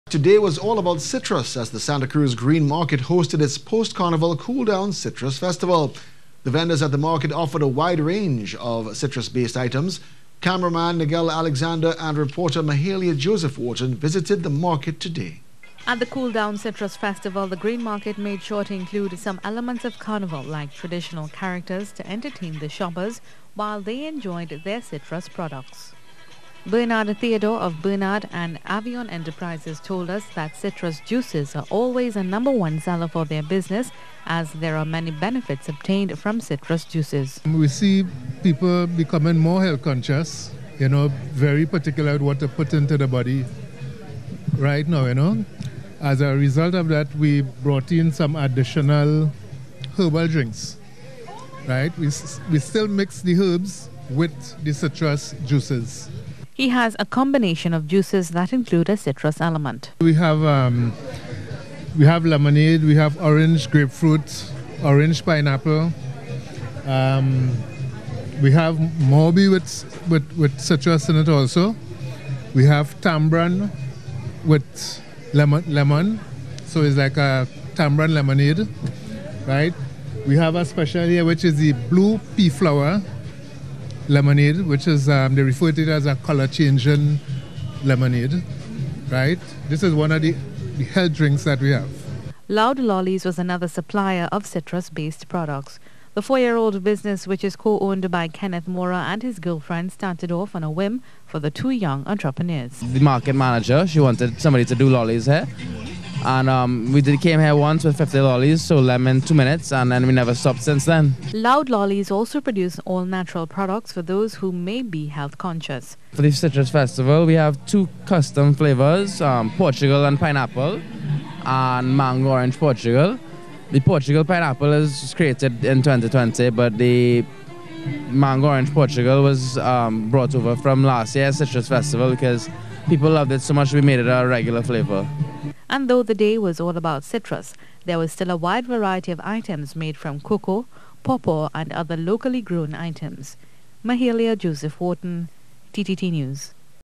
Santa Cruz Green Market Citrus Festival
It was all about Citrus as the Santa Cruz Green Market hosted its post-Carnival Cooldown Citrus Festival.

The vendors at the market offered a wide variety of citrus-based items.

Cameraman Negel Alexander and reporter Mahalia Joseph-Wharton visited the market.